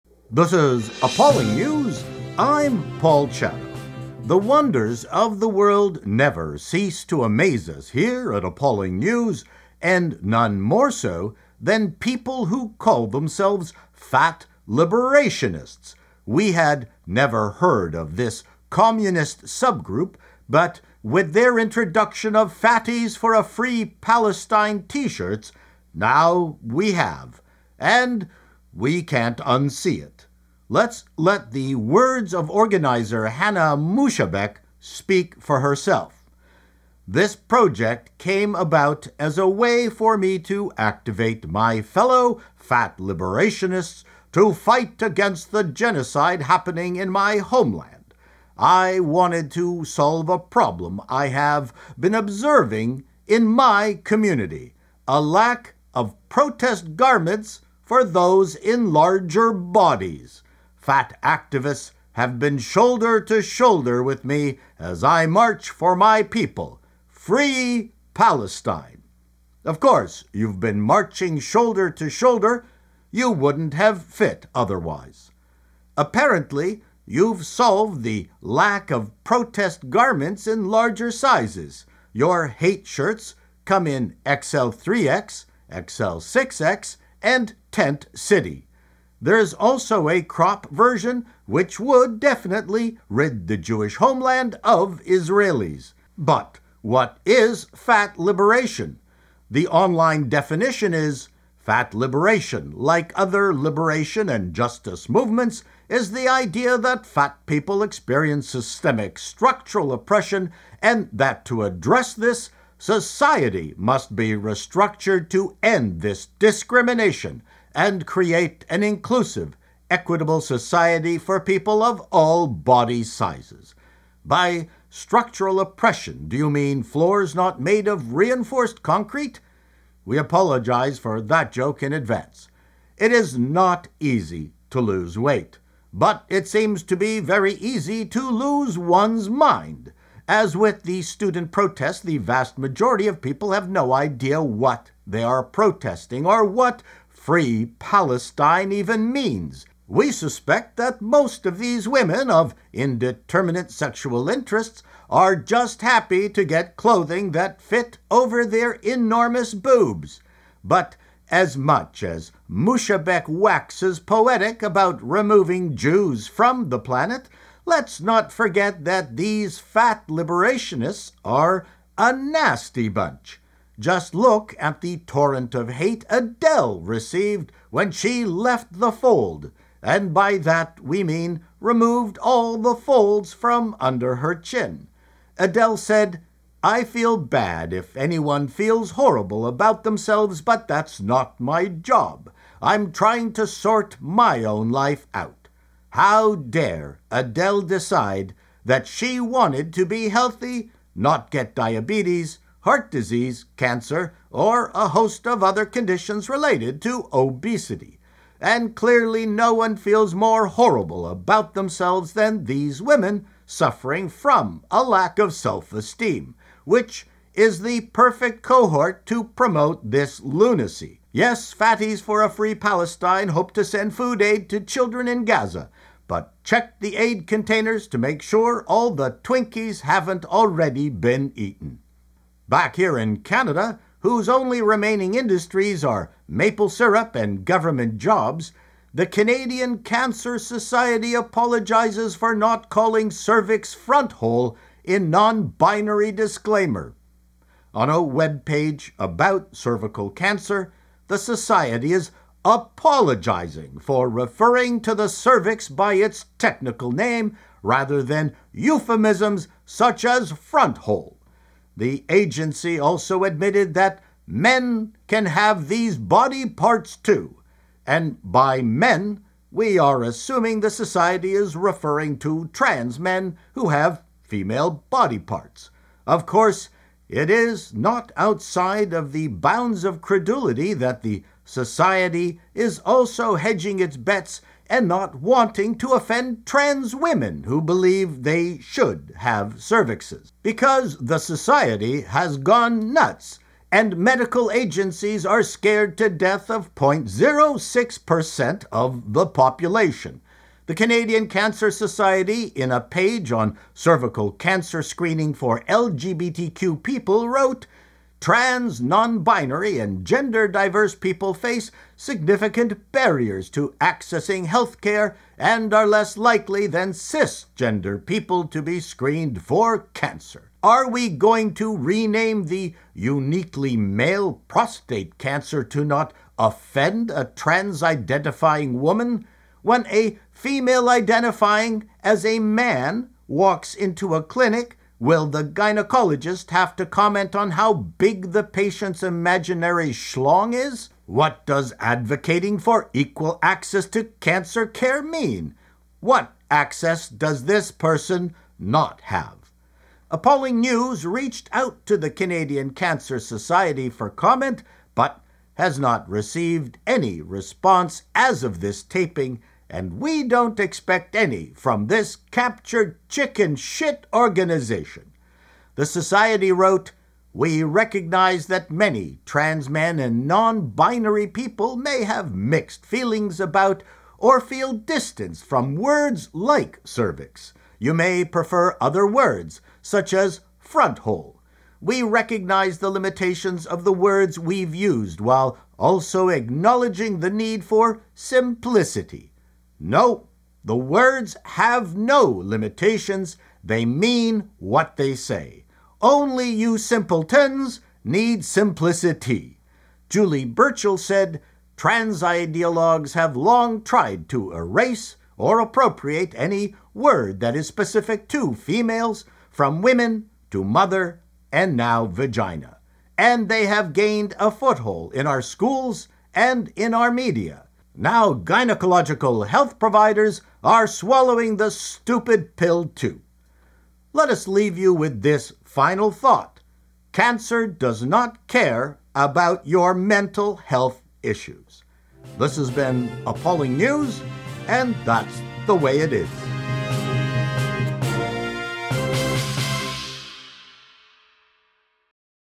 Fat Liberationists? Cancer Society apologies for using 'cervix'?
Just stuff I find amusing and horrific